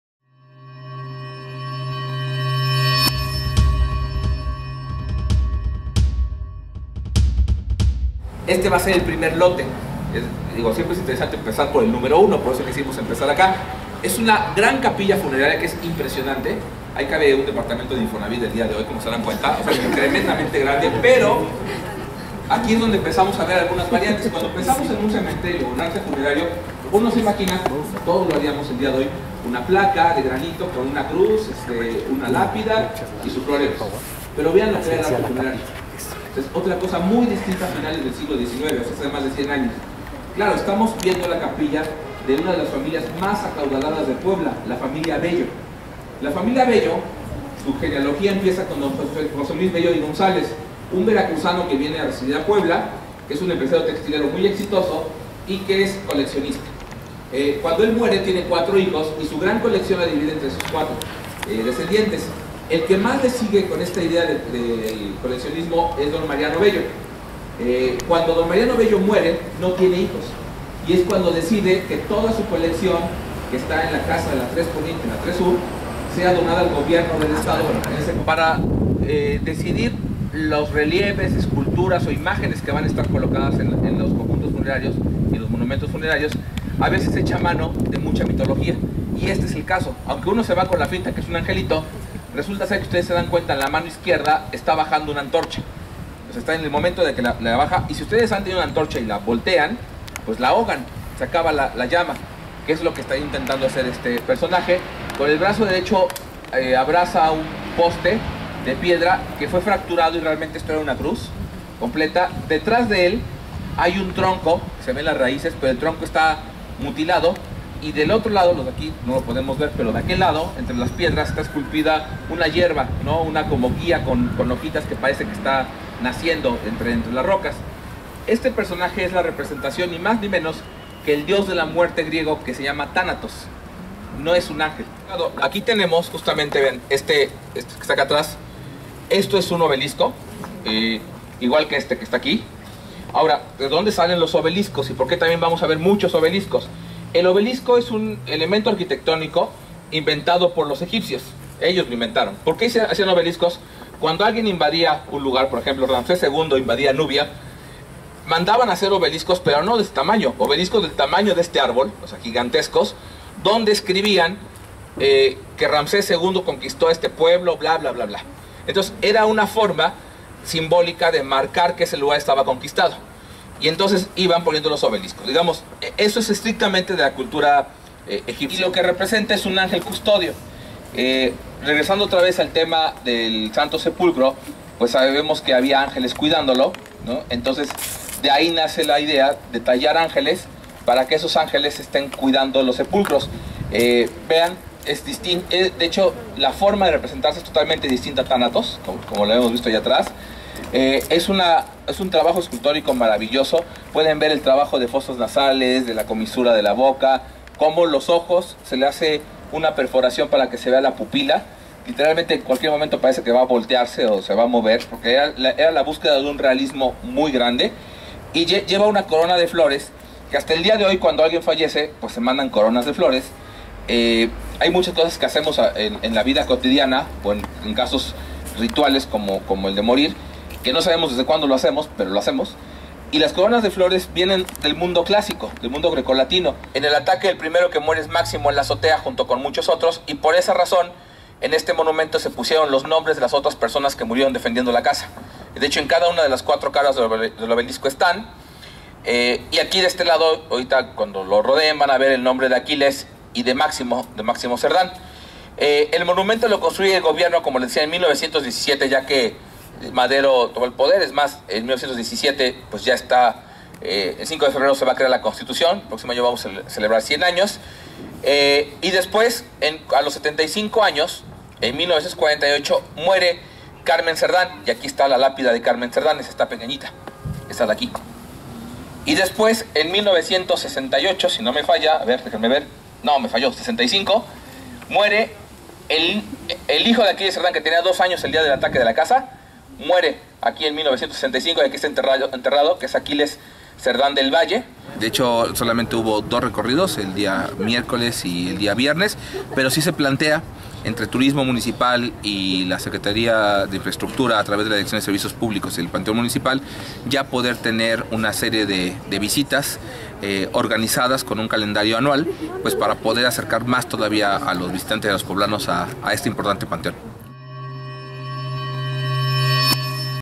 Recorrido nocturno en el Panteón Municipal de Puebla
Esto es parte de lo que se vivió en los dos “Recorridos Culturales, Hombres Ilustres y Arquitectura Funeraria”, en el interior del Panteón Municipal, ubicado en 11 sur y 35 poniente.
28 octubre 2016